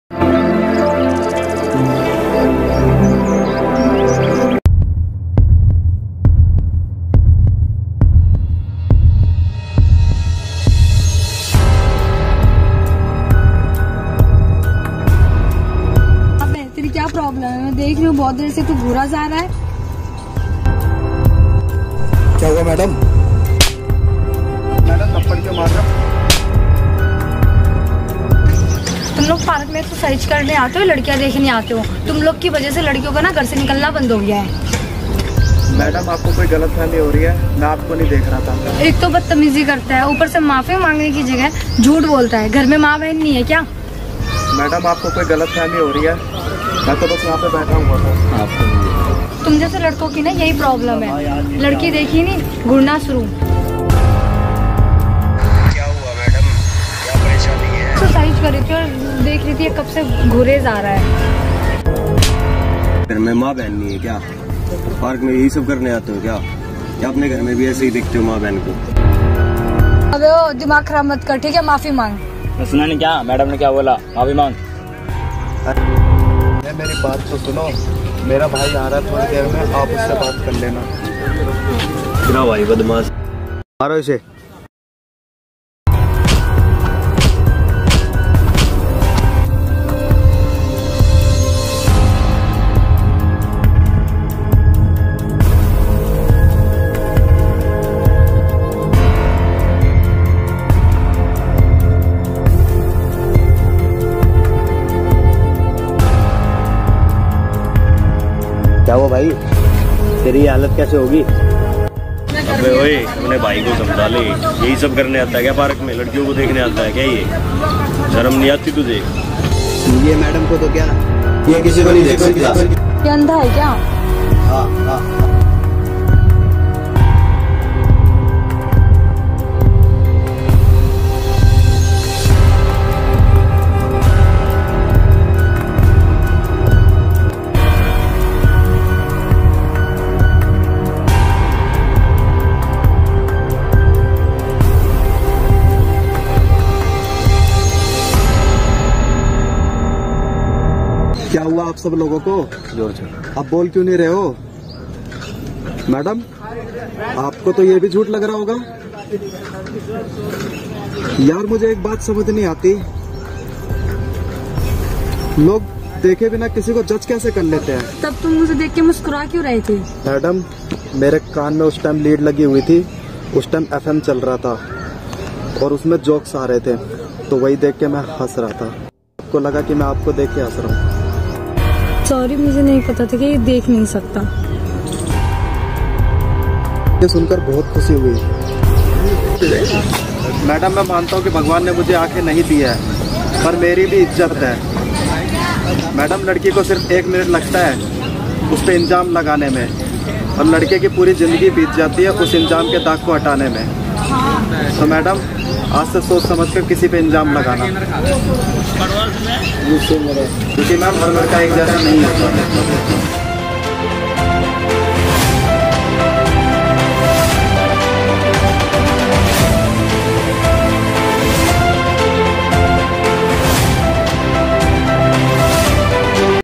GENDER EQUALITY - A GIRL FALSELY ACCUSED A BLINDMAN
FIlm BY HR BROTHERS